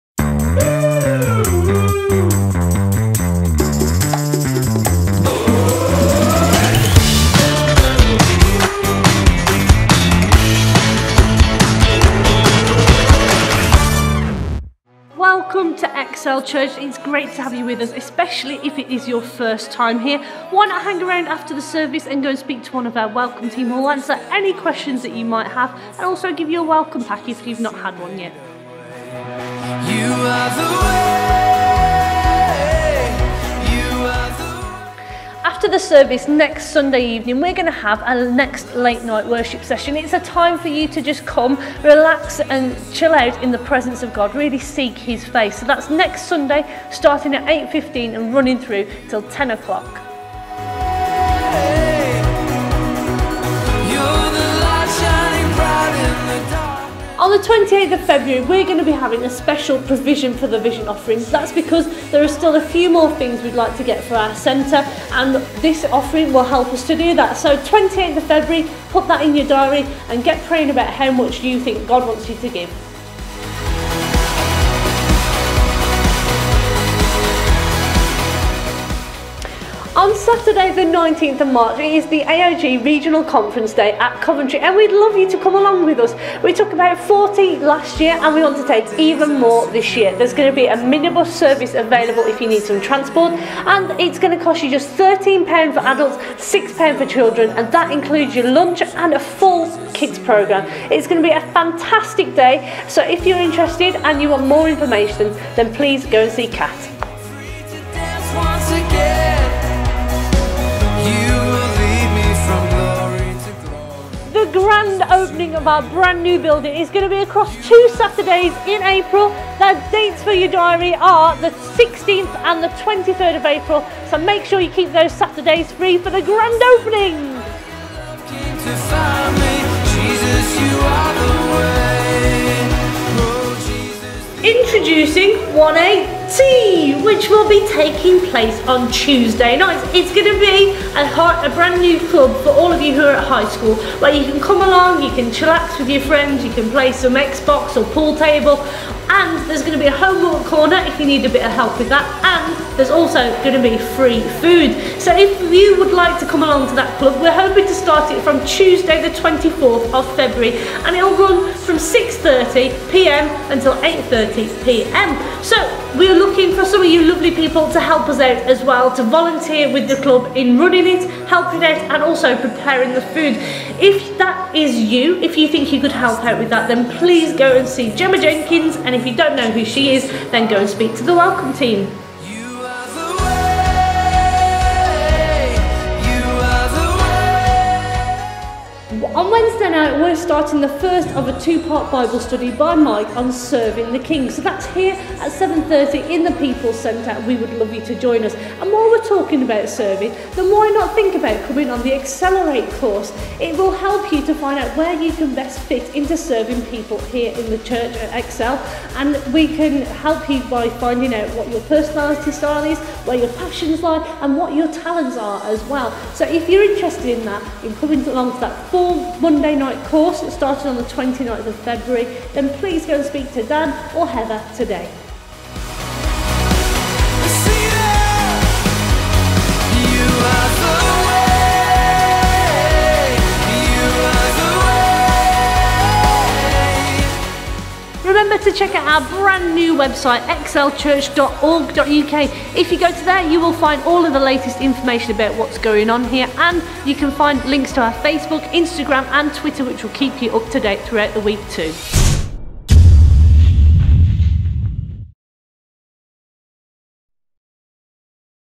Excel News (7th Feb 2016)
Our brand new weekly news programme for Excel Church. Find out what's coming up over the next few weeks and months here at Excel. Includes a brand new programme for high school students and details of the official opening of the new building!